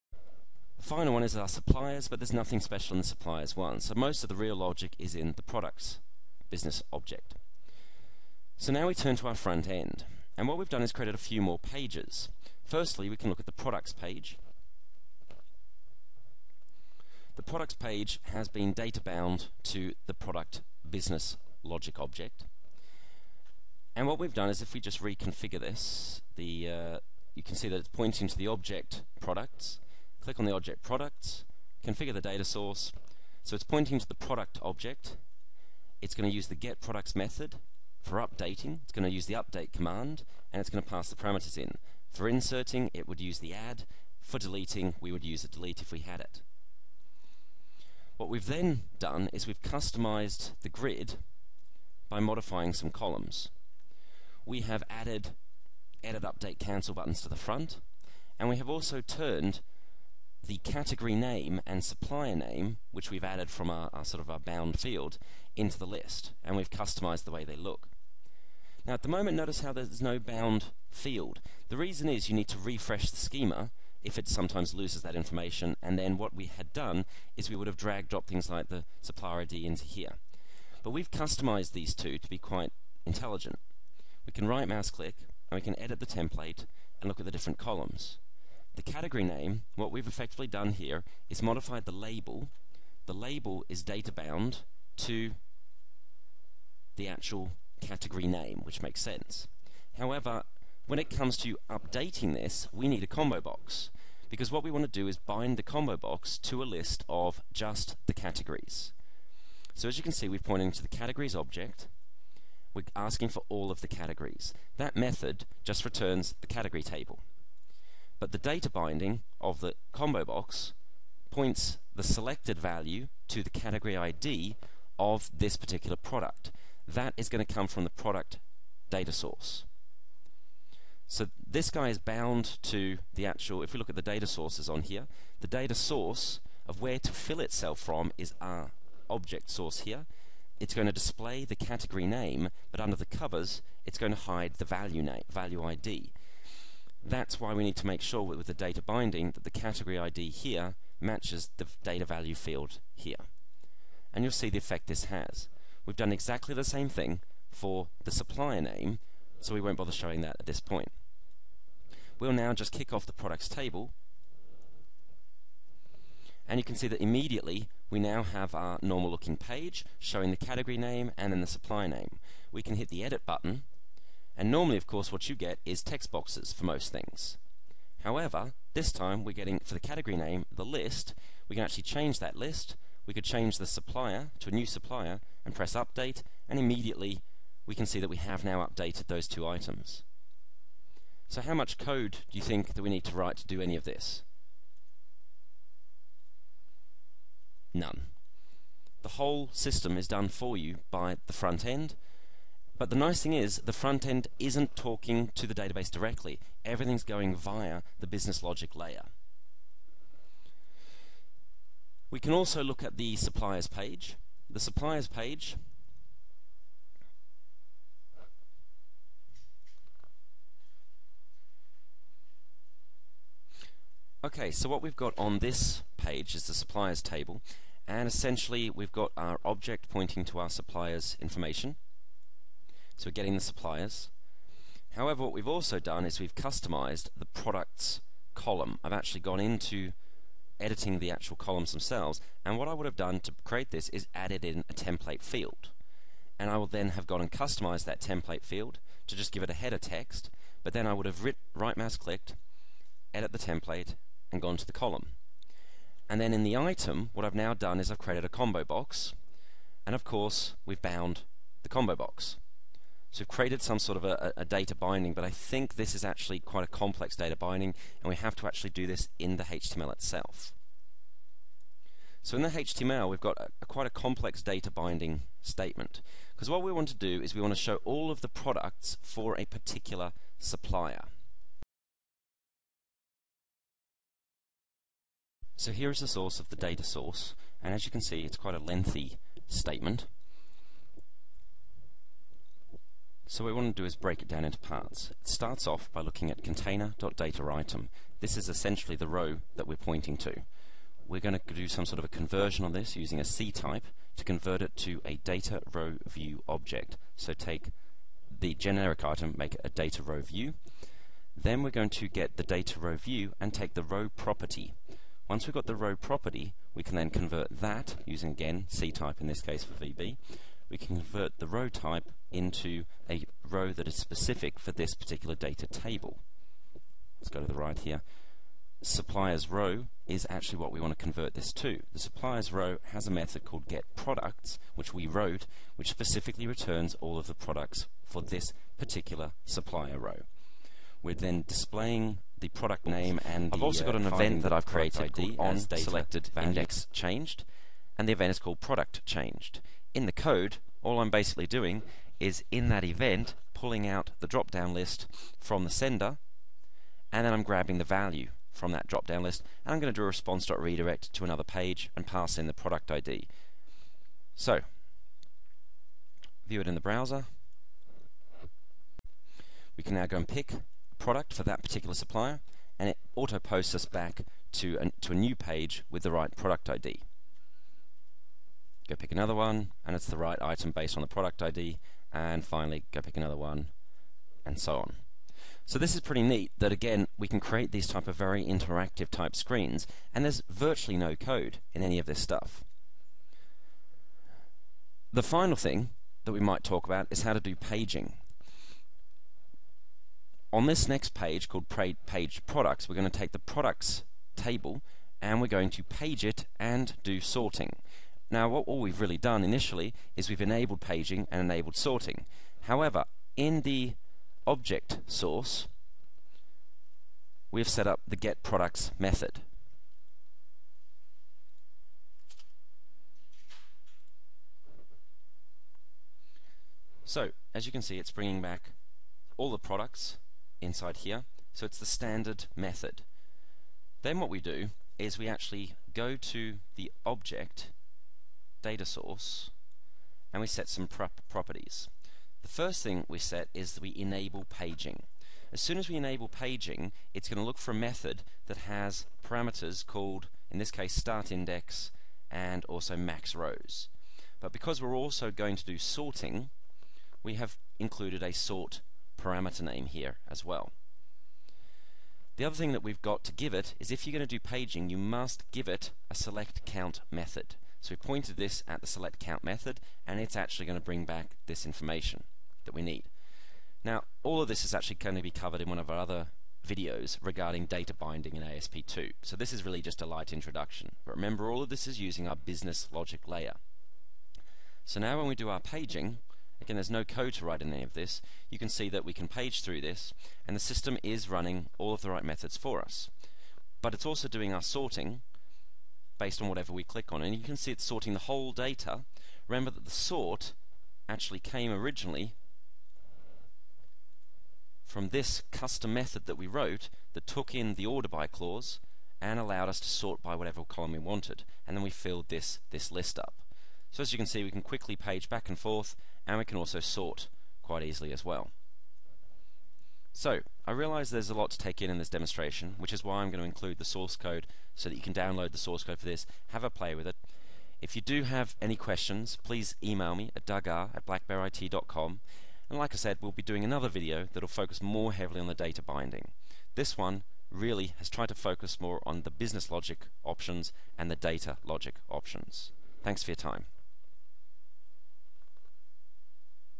Three Tier Data Access using ASP.NET 2.0 Part 6 of 6
Final part of this screen cast by Doug Rees from Black Bear IT presents how to create three-tier architecture for ASP.NET applications using datasets. However, this technique can also be used for windows forms applications. Although the video is created using Visual Studio 2005 and .NET 2.0, it can also be used in Visual Studio 2008 and 3.5.